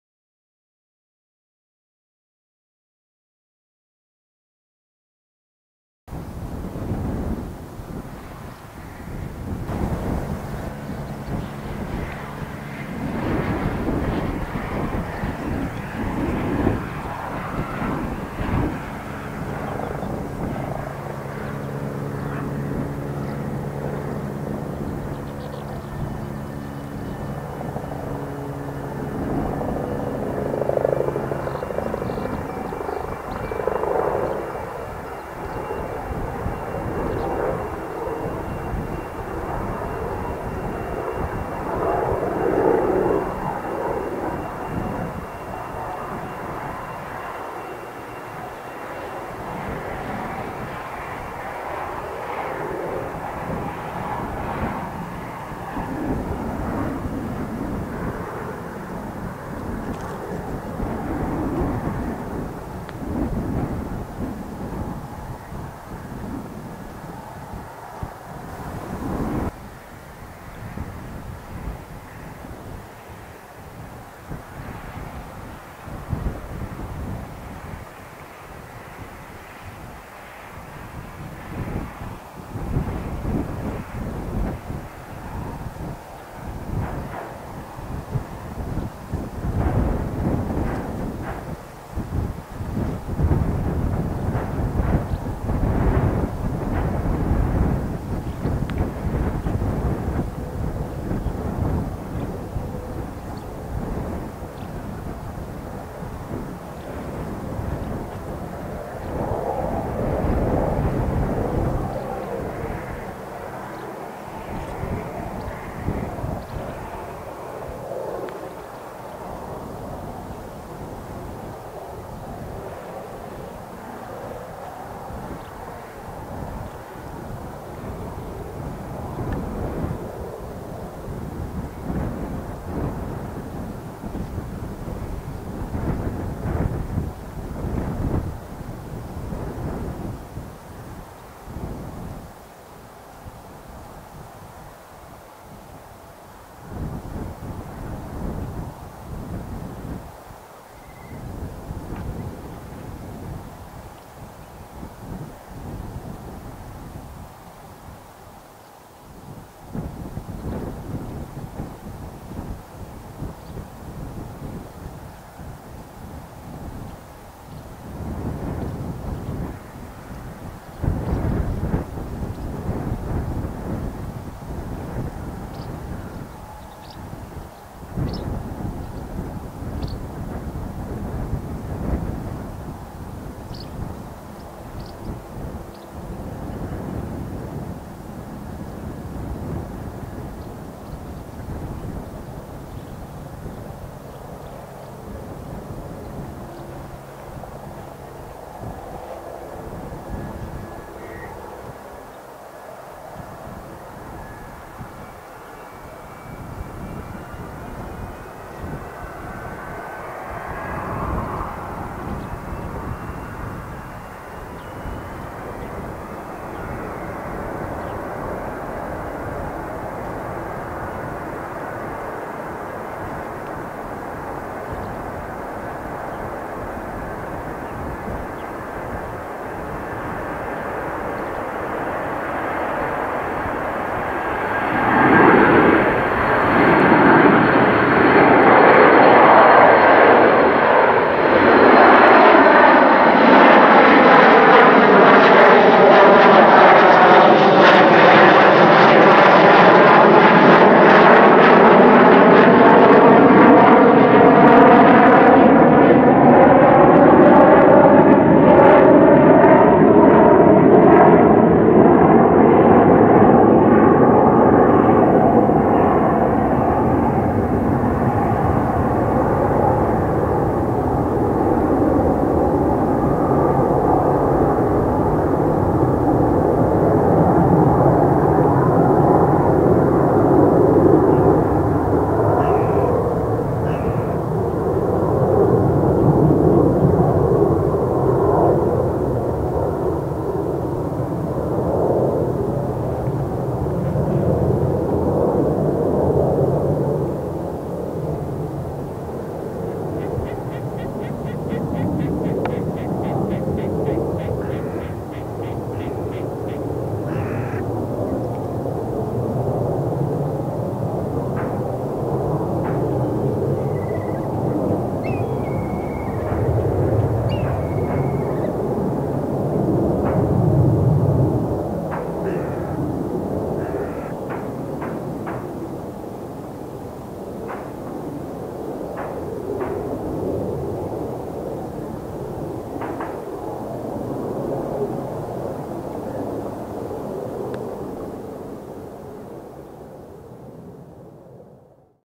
20231016 本日のブルーインパルス 支援機 C-1 離陸
ブルーインパルス支援機のC-1は
14:36ころ入間基地へ向け飛び立ちました。
晴れ。強風。